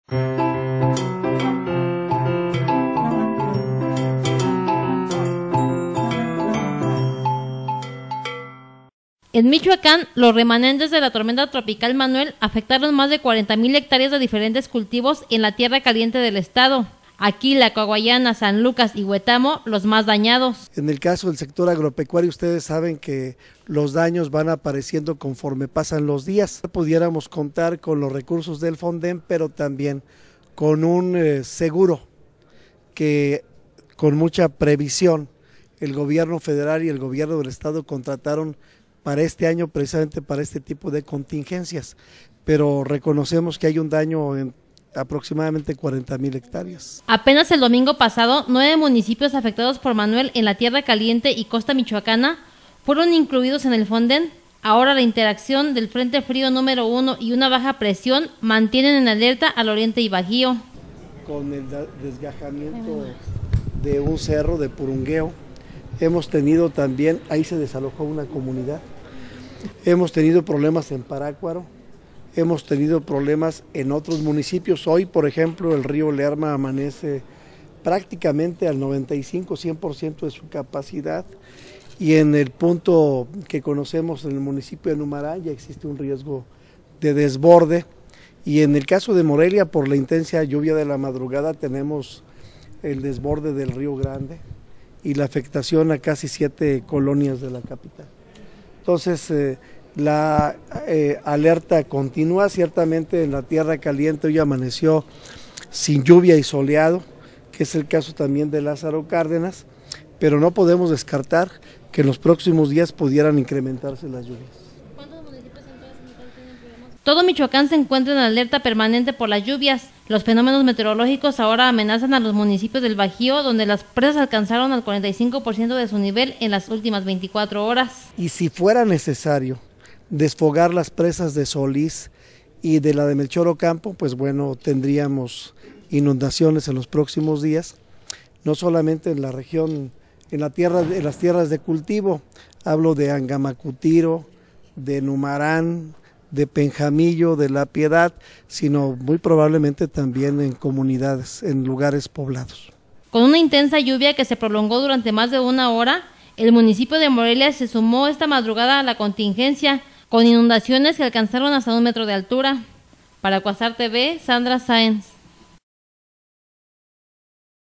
Más de 40 mil hectáreas de cultivos afectadas en Michoacán
En Michoacán, los remanentes de la tormenta tropical Manuel afectaron más de 40 mil hectáreas de diferentes culticos en la Tierra Caliente del estado, Aquila, Coahuayana, San Lucas y Huetamo, los más dañados...